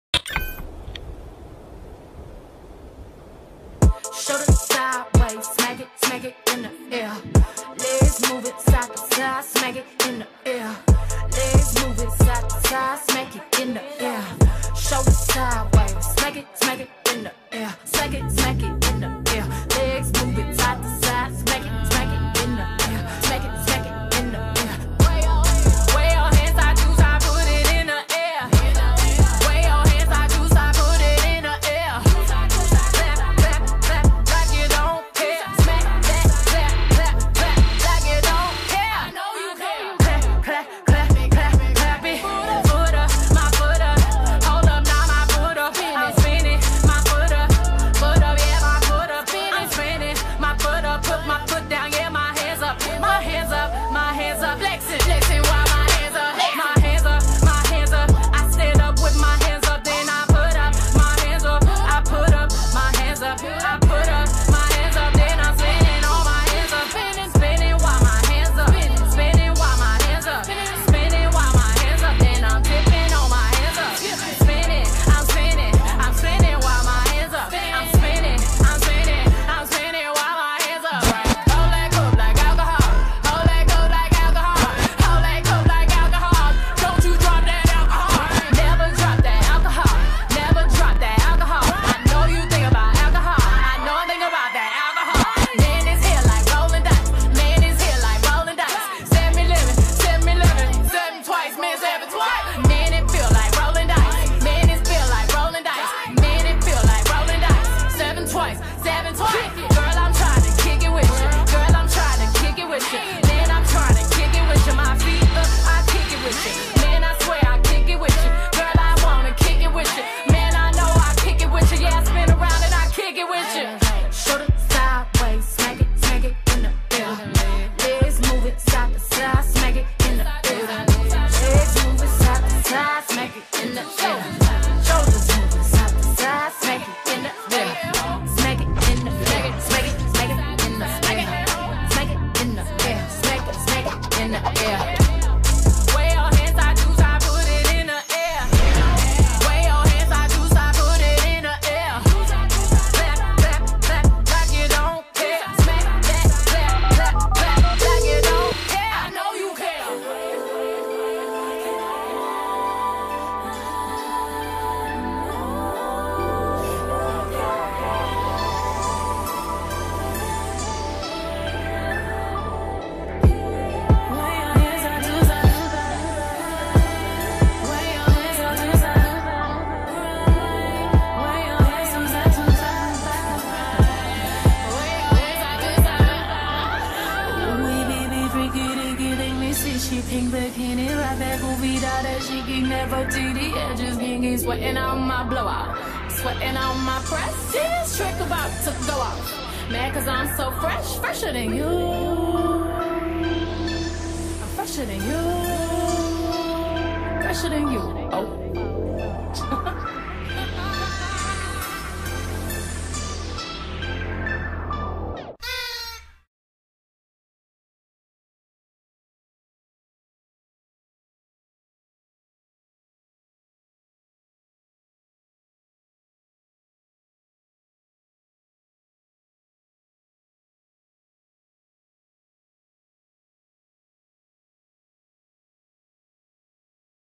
foot up hands up
this awesome song just made me stand up and try this lisa lottie's trick. okay now time to polish  it weeeeeeeeeeeeee!!!